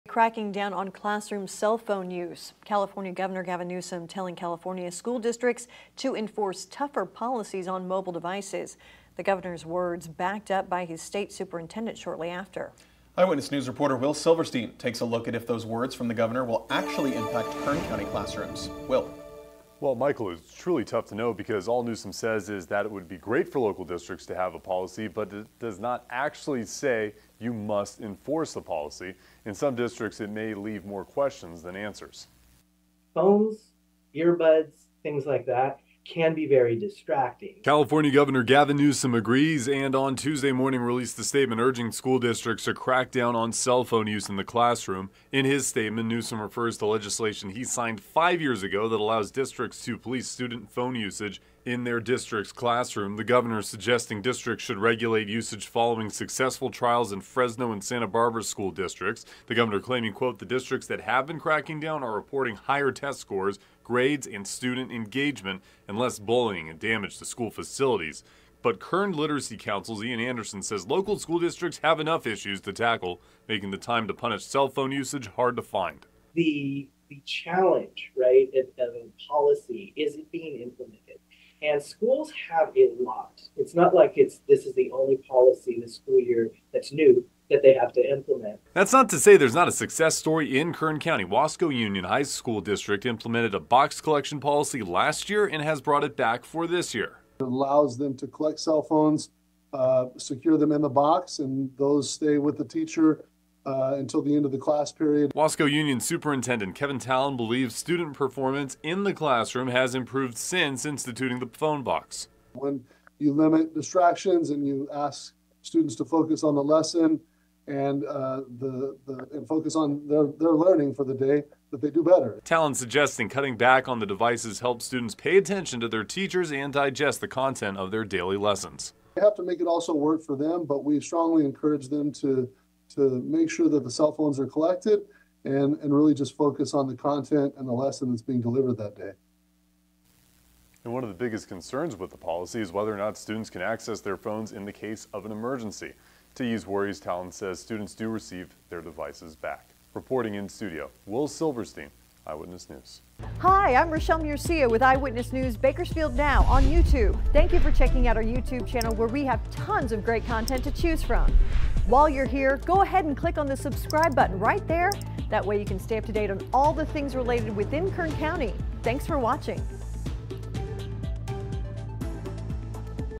Gov. Newsom urges school districts to implement phone restrictions
California Governor Gavin Newsom is urging school districts to crack down on classroom cell phone use. But it's not clear there is much the Governor can do to stop those texts.